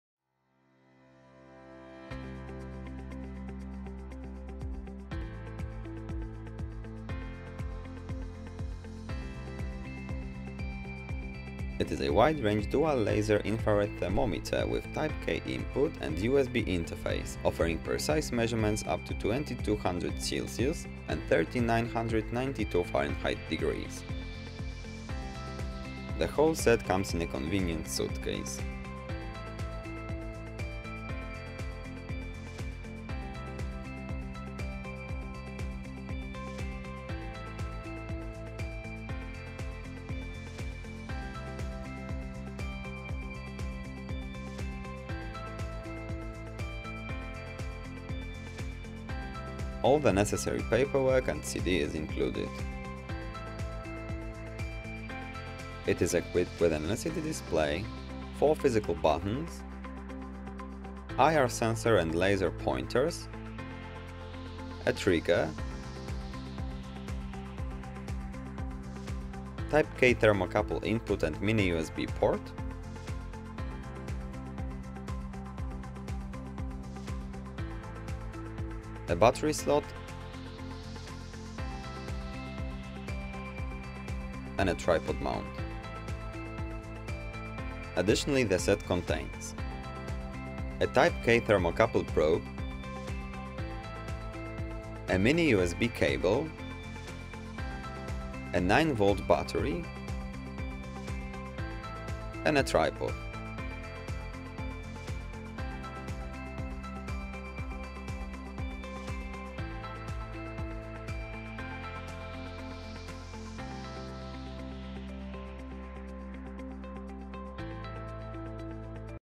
EXTECH 42570 - Infrared thermometer - UNBOXING
Type of meter: infrared thermometer
TME Symbol: EX42570

Standard equipment:
• battery
• hard case
• K-type temperature probe
• software
• stand
• USB cable

Additional information:
• Gross weight: 1232.8 g
• Guarantee (months): 24